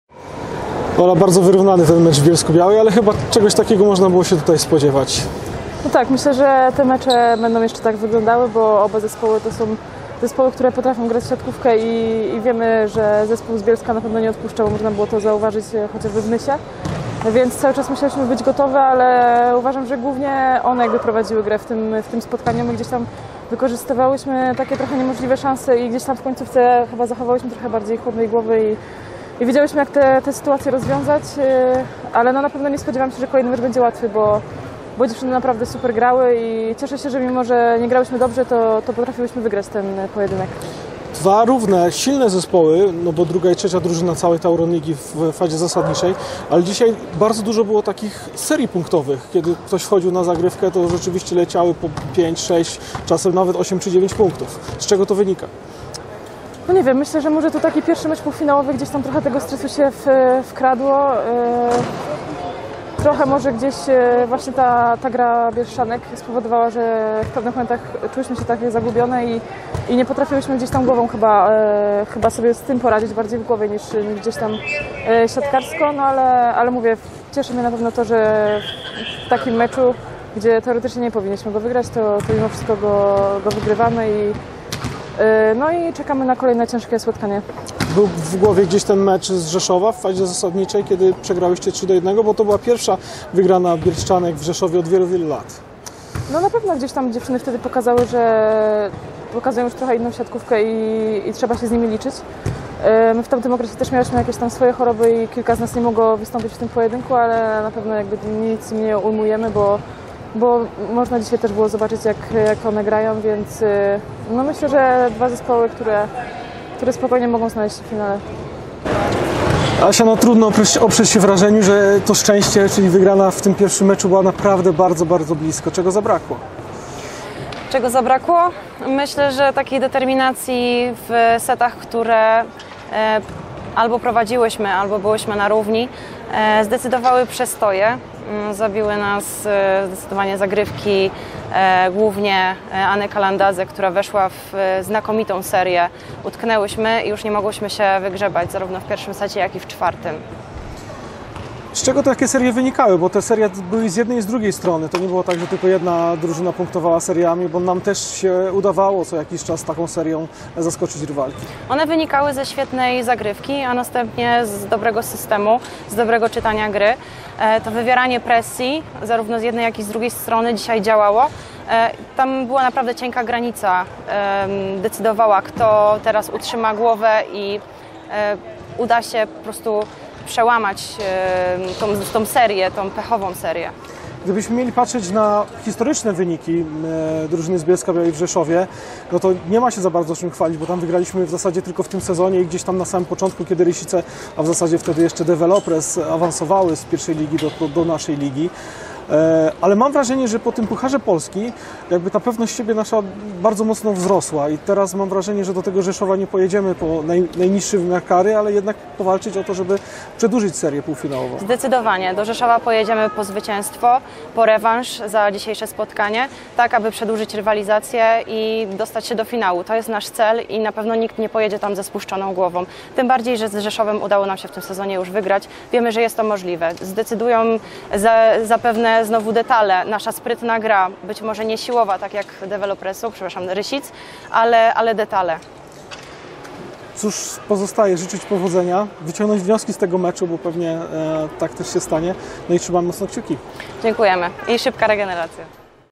Półfinał Mistrzostw Polski. Przegrana z Rzeszowem po tie breaku.
Aleksandra Szczygłowska i Joanna Pacak po pierwszym meczu półfinału Mistrzostw Polski.